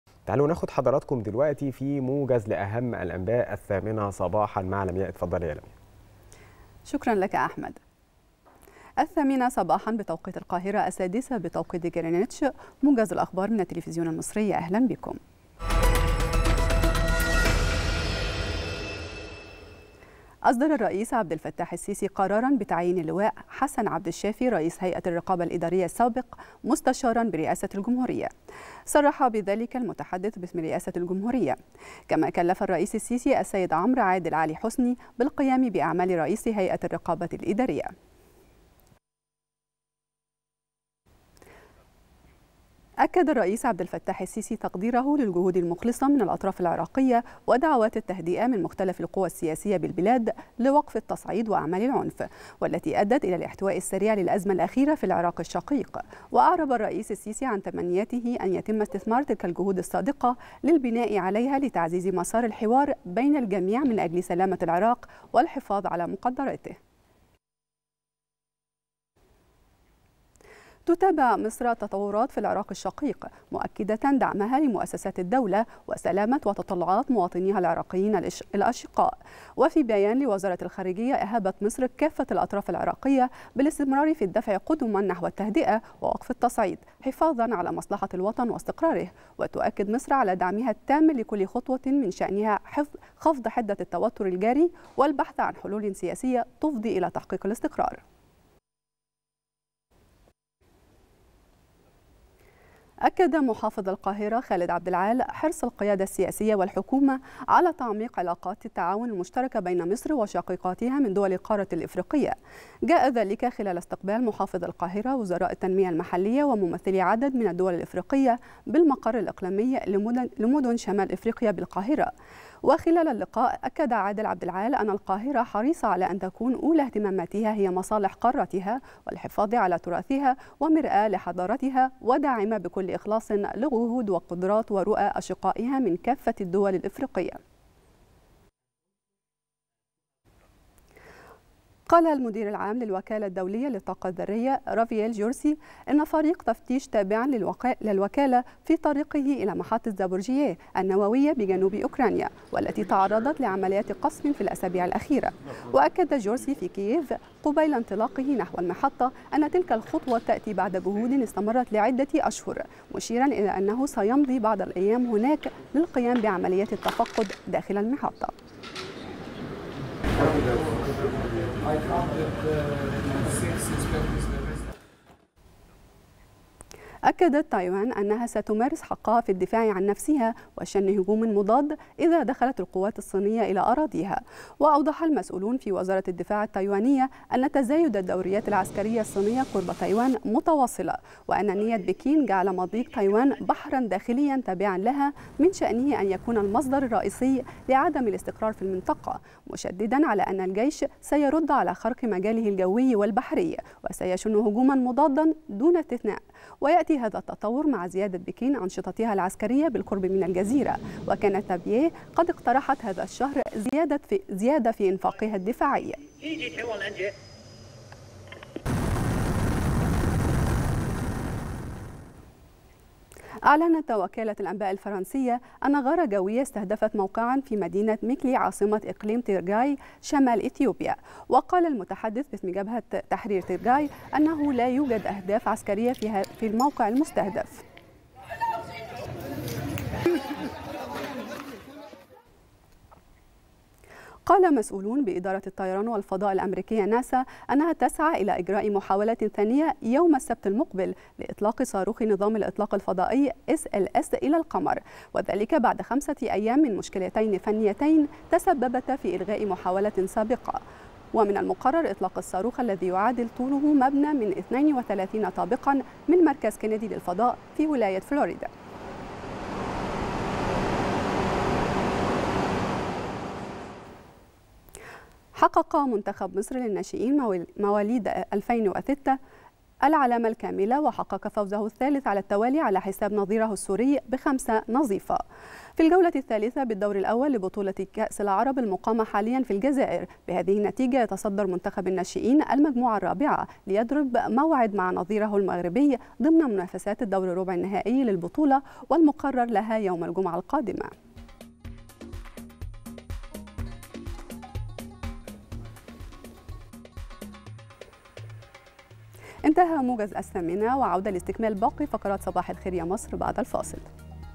الرئيس السيسي يصدر قراراً بتعيين رئيس هيئة الرقابة الإدارية السابق مستشاراً برئاسة الجمهورية
موجز الثامنة.. الرئيس السيسي يصدر قراراً بتعيين رئيس هيئة الرقابة الإدارية السابق حسن عبد الشافي مستشاراً برئاسة الجمهورية